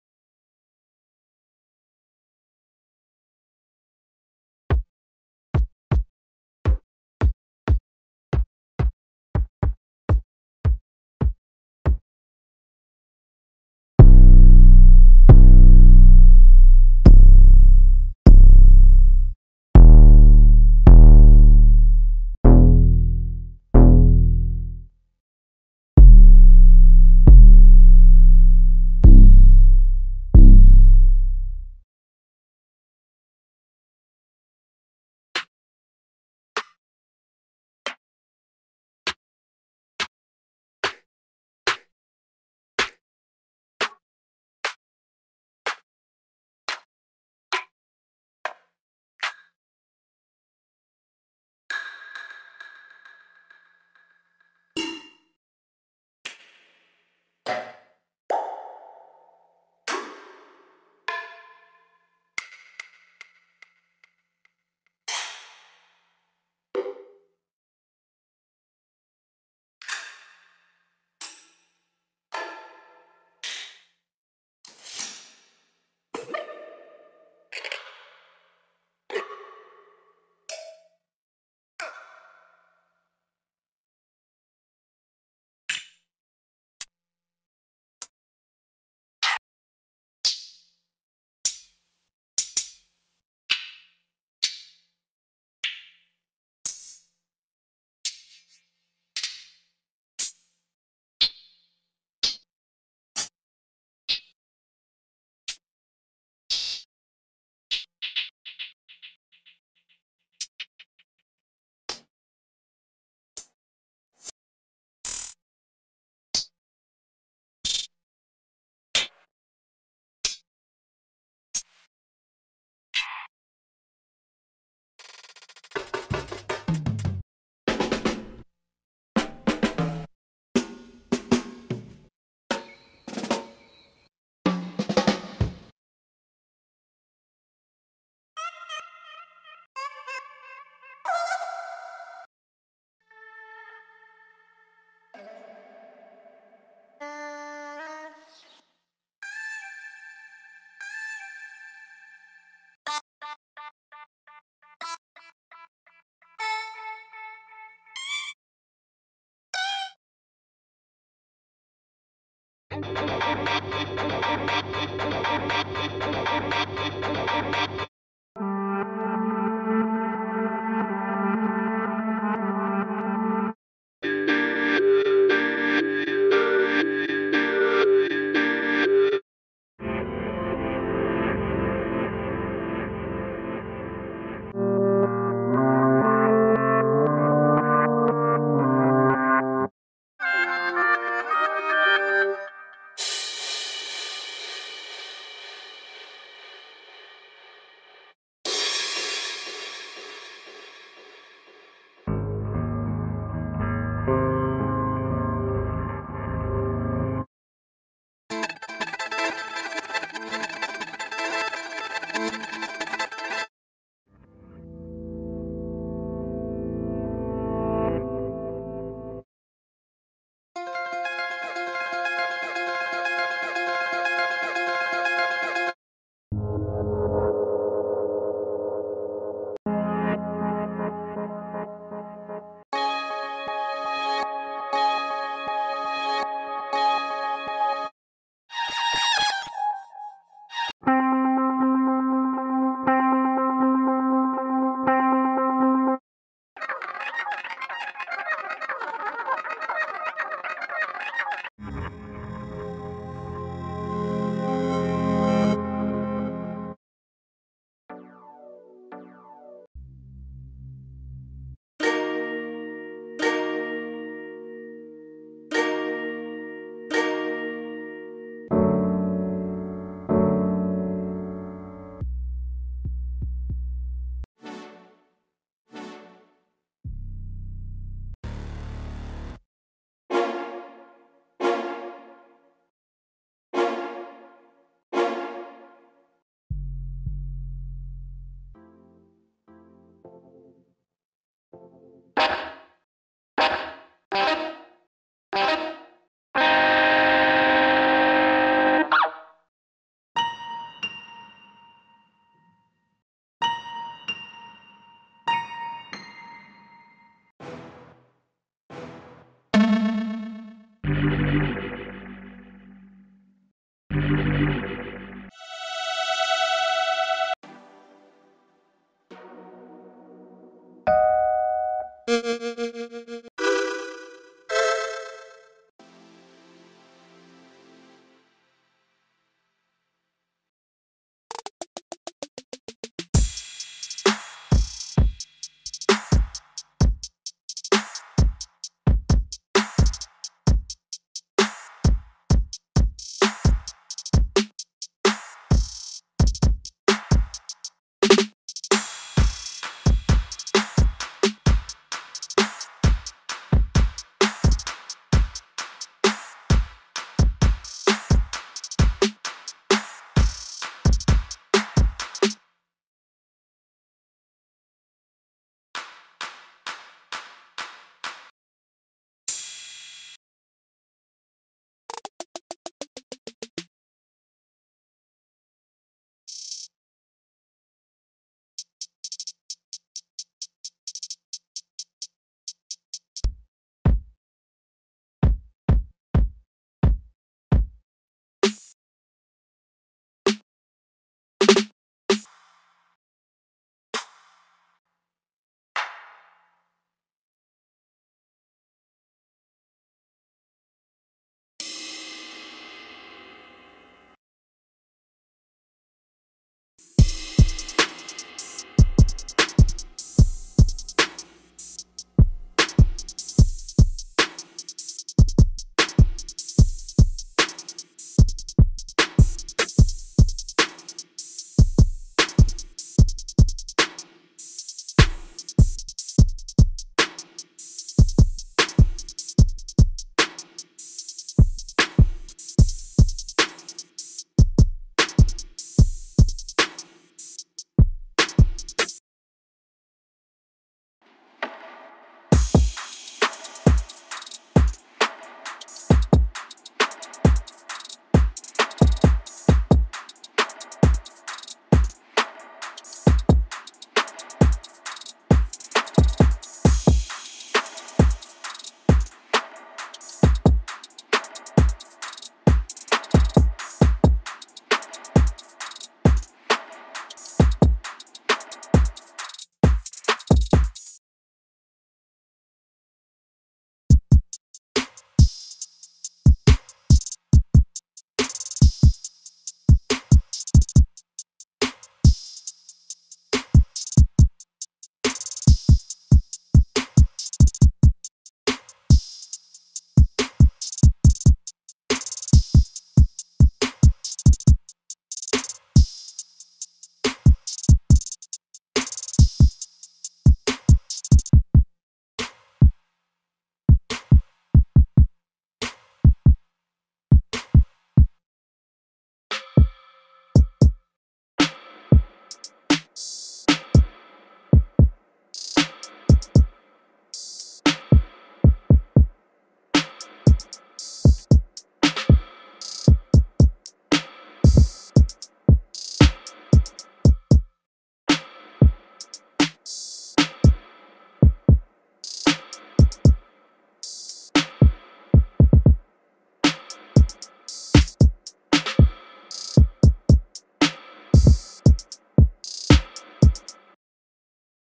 One shot kit + Trap Drum kit 🔥 METAMORPHOSE 🔥
free one shot kit + trap drum kit  - METAMORPHOSE

🔥 Dust off your speakers and start producing some hard-hitting Trap beats with this top-notch drum production toolkit! Included is an arsenal of thrilling drum loops, ranging in BPM and key, all broken down into stems, and almost 15 punchy drum one shots. Dusty kicks, crunchy snares, crispy hats, booming 808’s, and electrifying percussive elements, all designed to help you take your productions to new heights and get the most out of your tracks. Get cracking with this high-end sample pack and get your Trap beats popping! 🔥

……:::::: Specifications ::::::……

• 6 x 808s
• 10 x Kicks
• 16 x Snares
• 20 x Percussion
• 30 x Tops
• 06 x Drum Fills
• 05 x Drum loops
• 23 x Percussion & Glitch Loops
• 12 x vox
• 19 x FX
• 28 x Melodic Oneshots
• Compatible With All DAWs
• PC & Mac Compatible

🎬 In this link you will find more loops and trap libraries.